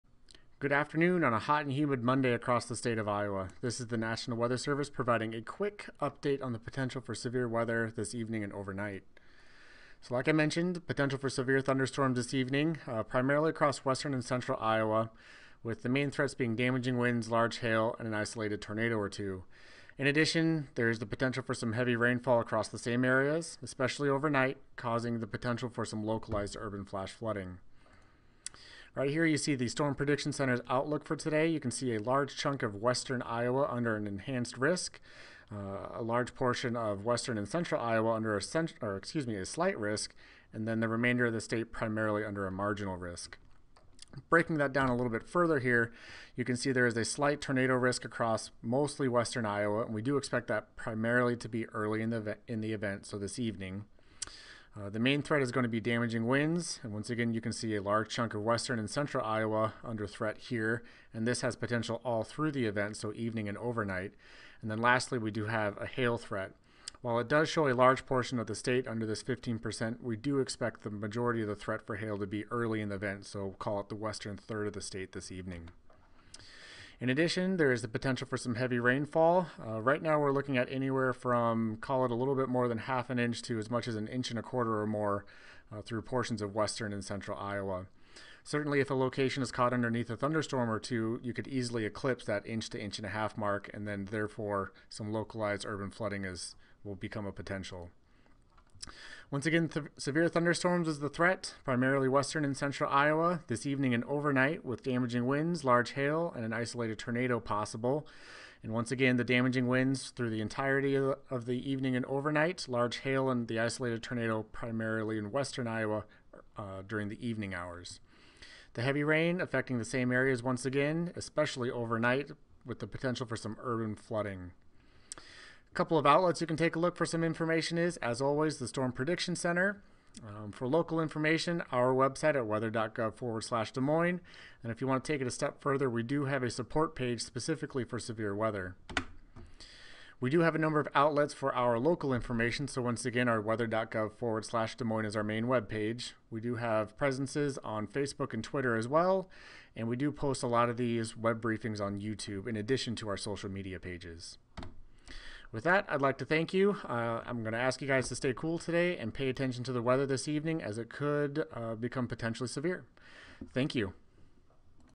NWS Weather Briefing for July 11, 2016
NWS Des Moines weather briefing for forecast severe weather on July 11, 2016, including potential for damaging winds, large hail, and a tornado or two.